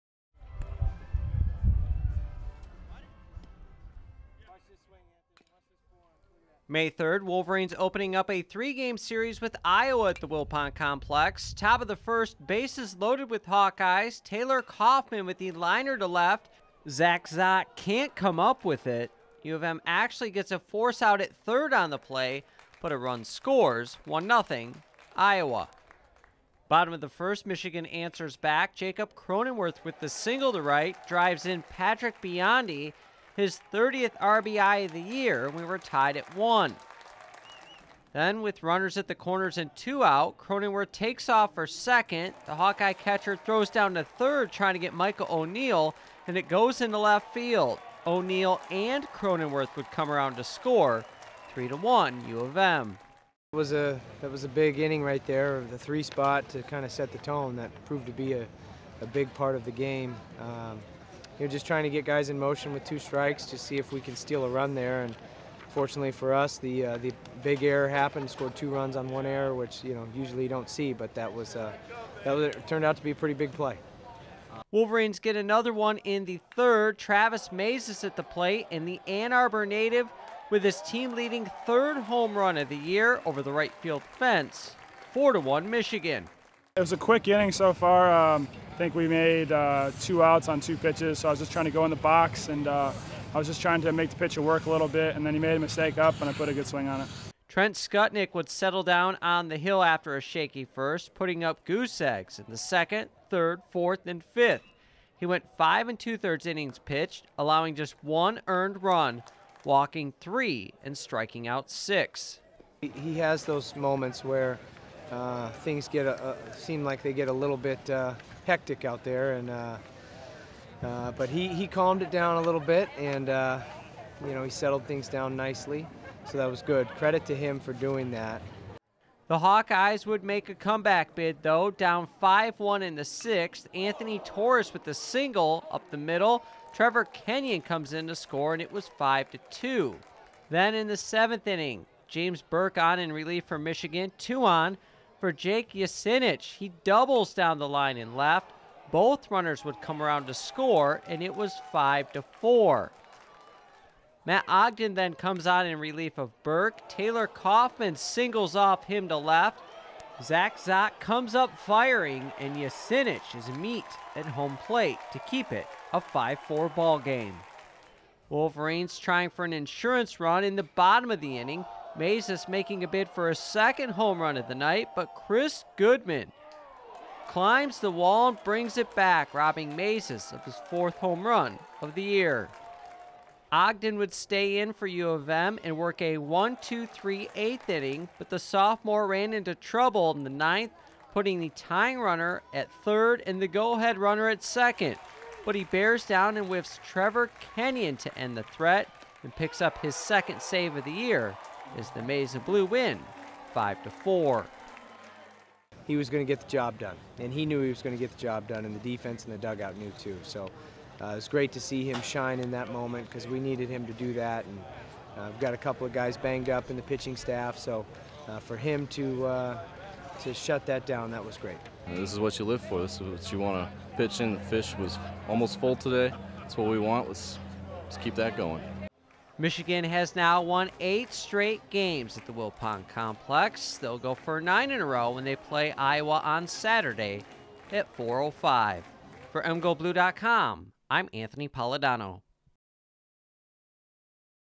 Michigan Baseball Uses Early Runs to Stop Iowa
U-M scored three times in the first inning, had a solo home run from Travis Maezes and struck out nine Hawkeyes in a 5-4 victory, extending its home win streak to eight.